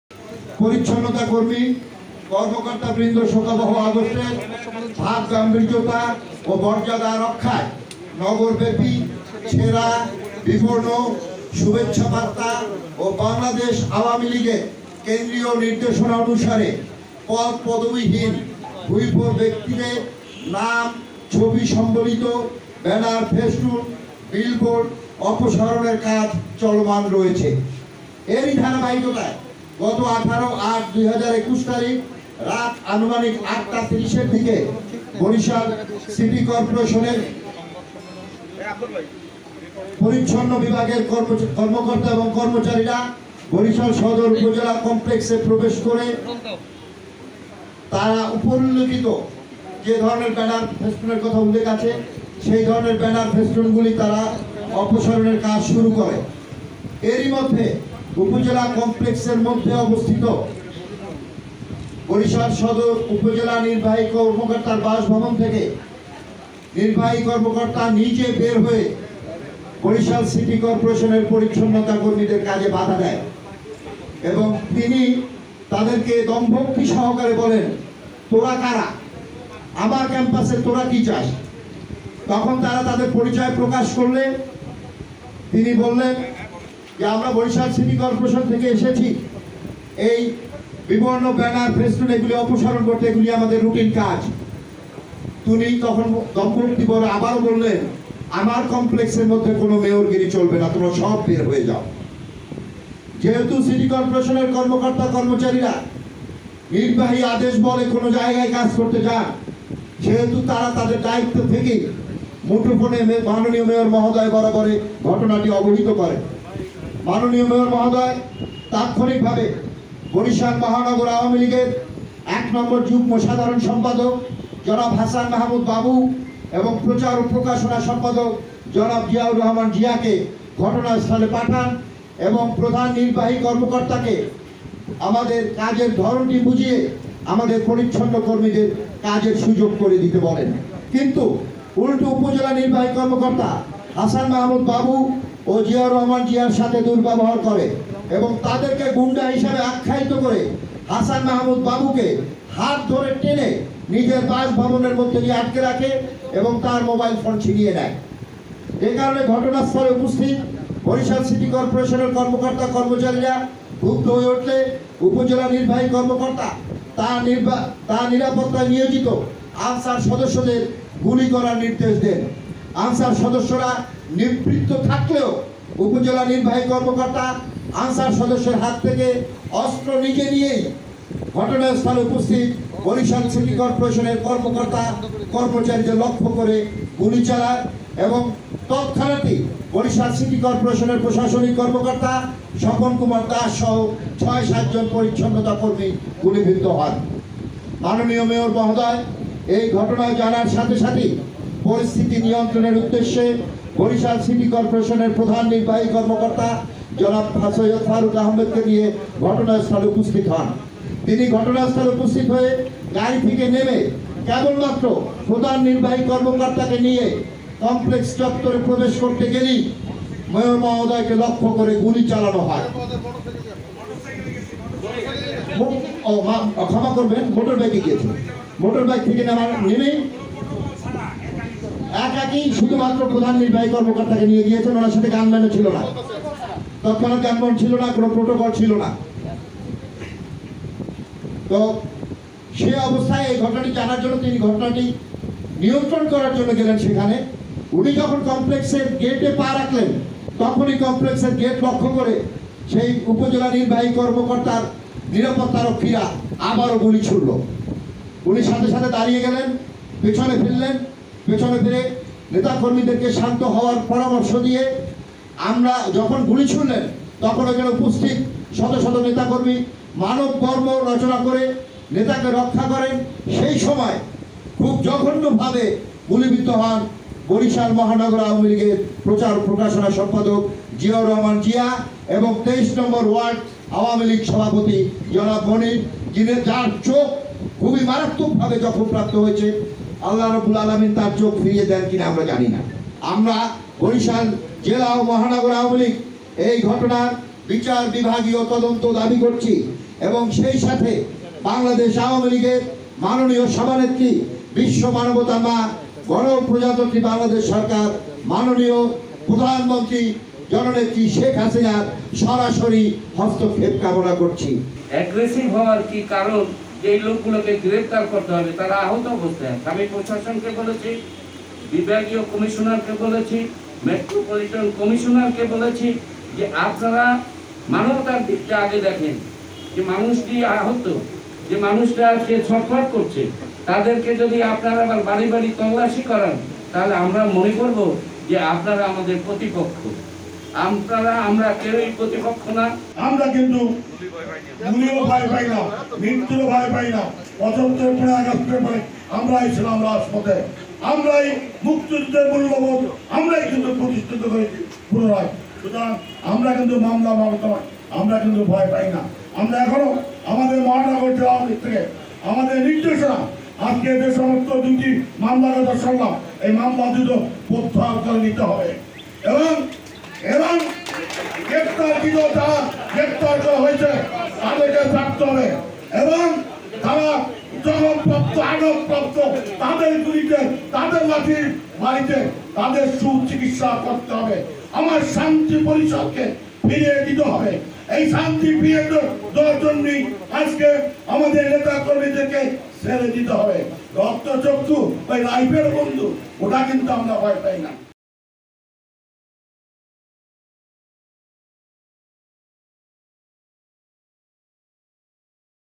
ইউএনওর নির্দেশে গু‌লি চালায় পুলিশ: ব‌রিশাল আওয়ামী লীগ | NewsBangla24
উদ্দেশ্যপ্রণোদিতভাবে আওয়ামী লী‌গ নেতা-কর্মী‌দের ওপর গুলি ছোড়ার নি‌র্দেশ দি‌য়েছেন ইউএনও মু‌নিবুর। এ‌তে...